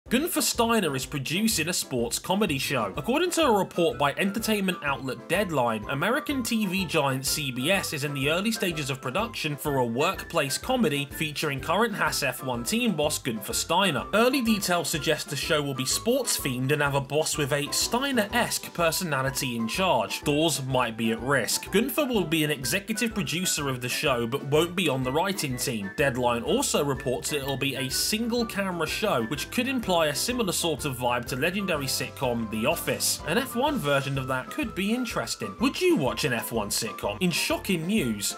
We don't know what to make of this 😅
Guenther Steiner is producing a TV Show!

The Haas team boss is reported to be producing a single camera sitcom featuring a "Steiner-esque" boss in charge. Will you be watching?

Follow WTF1